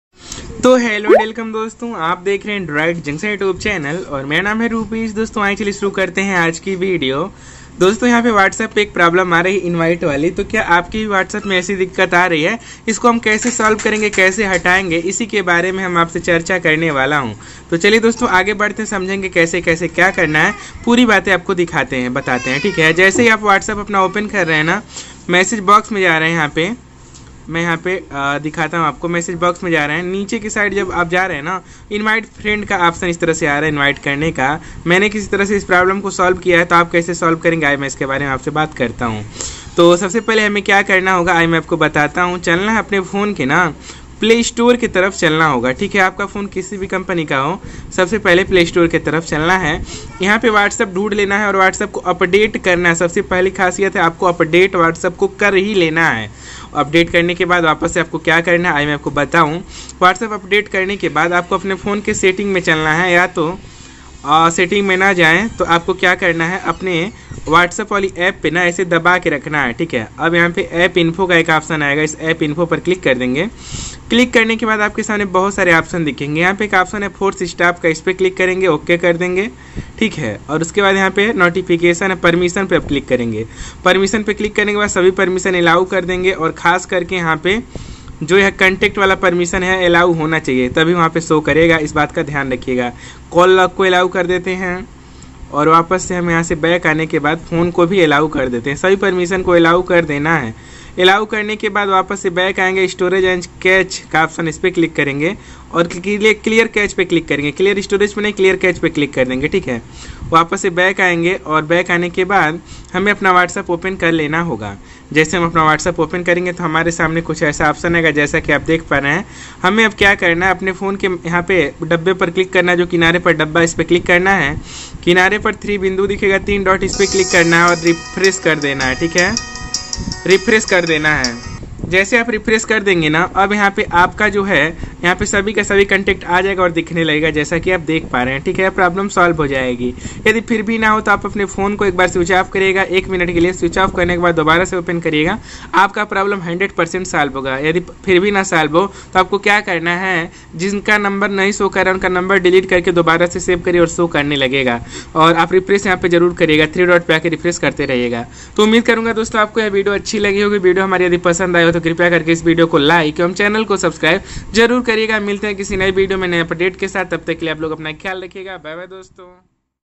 whatsapp invite kaise hataye !! whatsapp invite problem fix !! invite kaise hataye whatsapp problem
hello dosto kaise hai aap loag video me hm loag baat karenge ki whatsapp invite problem ko kaise thik kare ya whatsapp invite ko kaise hataye

Your Quieres 👇👇

whatsapp invite problem solution
whatsapp invite kaise hataye
whatsapp invitation remove kaise kare
whatsapp invite problem ko kaise hataye